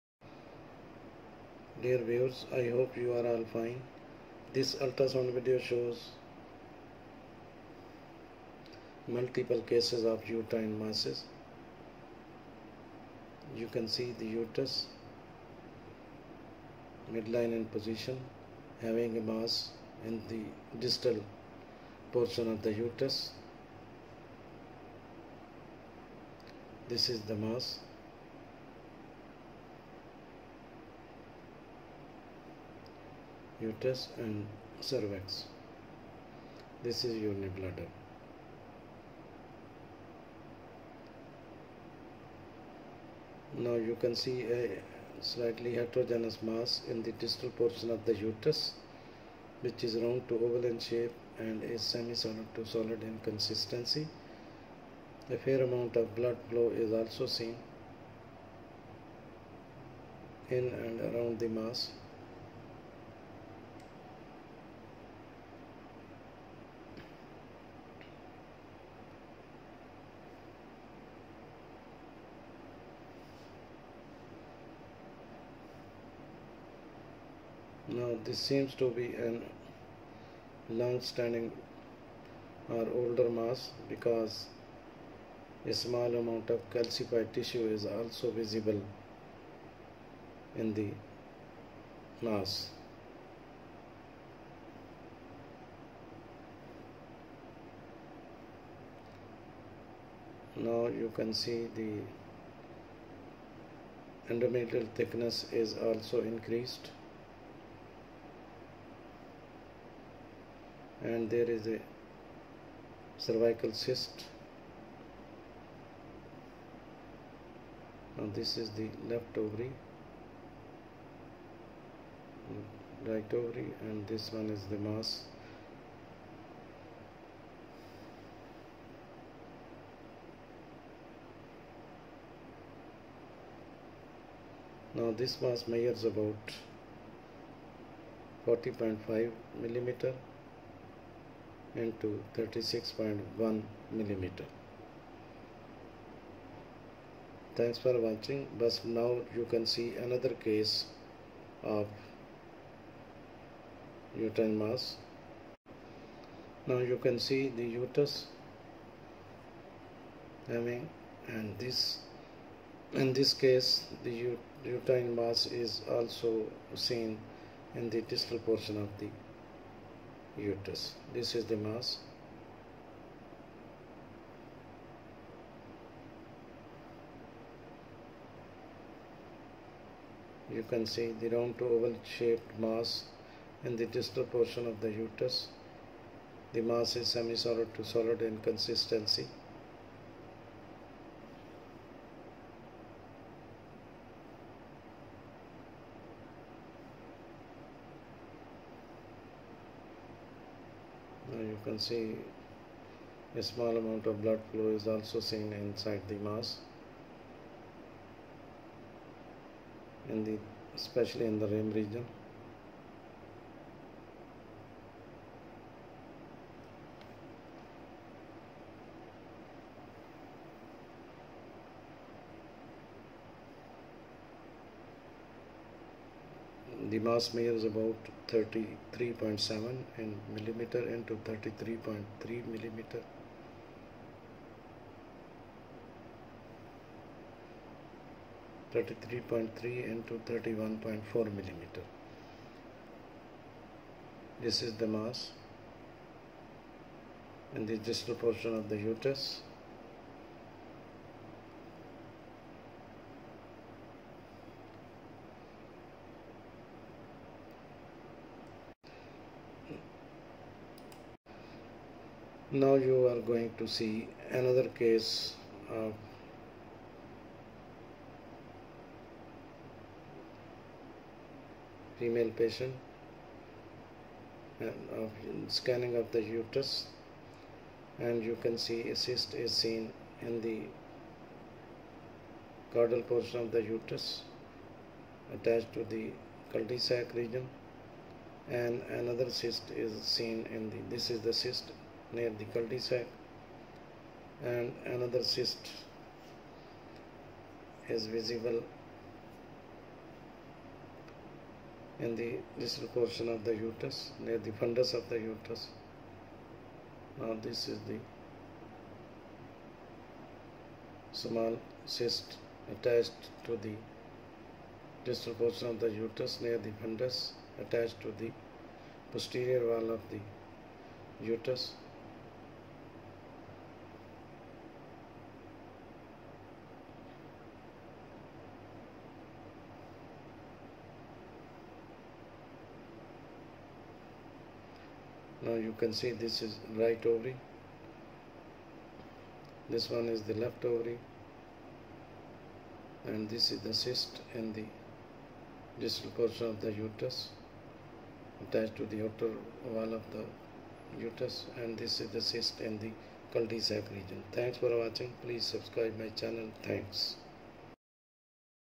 Ultrasound Video showing Multiple cases of uterine mass and cyst.
This video shows Multiple cases of uterine mass and cyst.